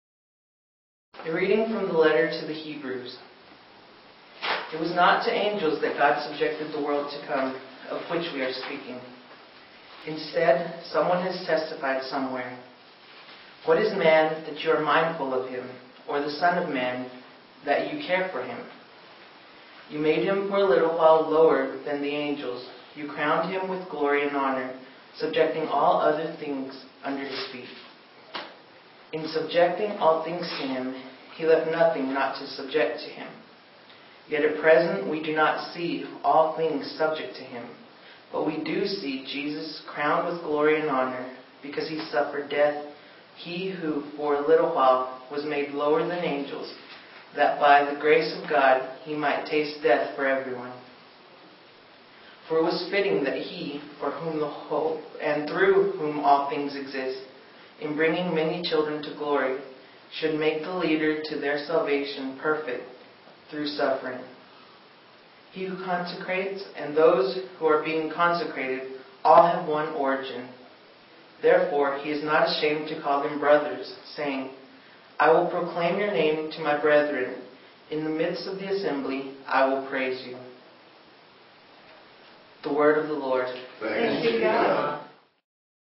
01/11/11 First Reading
Lucas Flores reads the First Reading for today, Tuesday of the First Week In Ordinary Time, from the Our Lady of Gudalupe chapel at the Catholic Center.